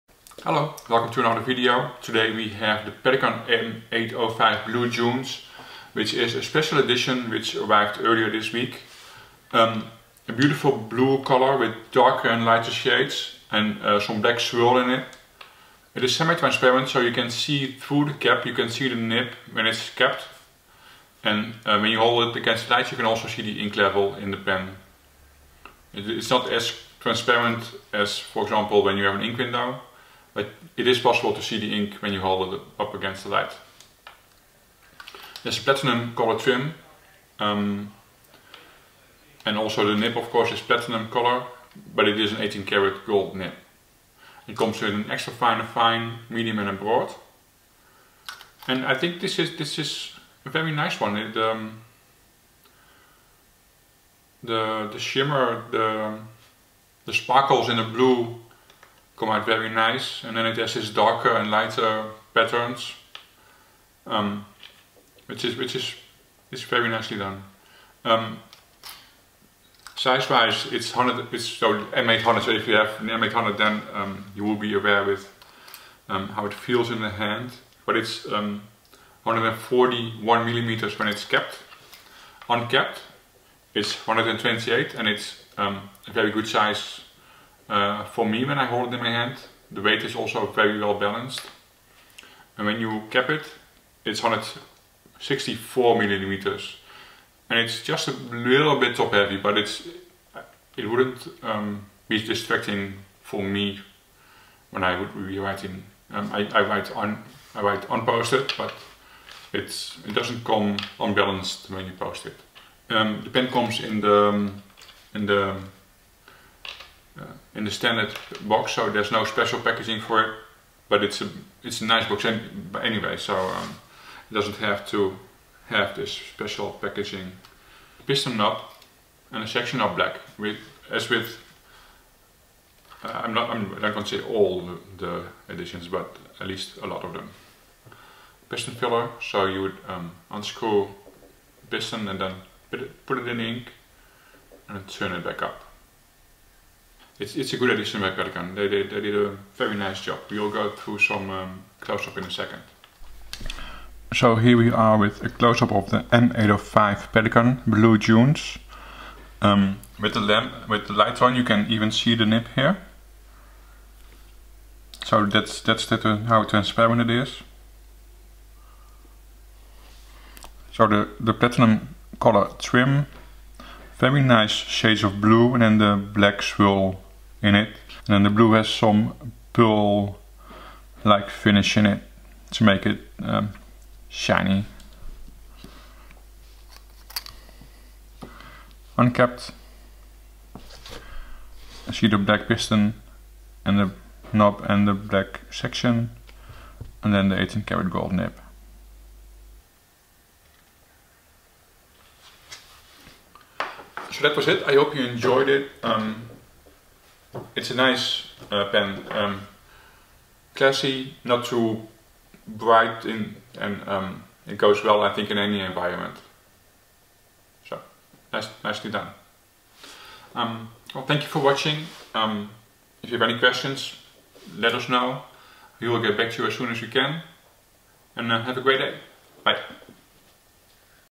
Pelikan M805 Blue Dunes
Pelikan M805 Blue Dunes fountain pen
In this video we show details of the Pelikan M805 Blue Dunes fountain pen. The Blue Dunes is a special edition, launched in Jumne of 2019.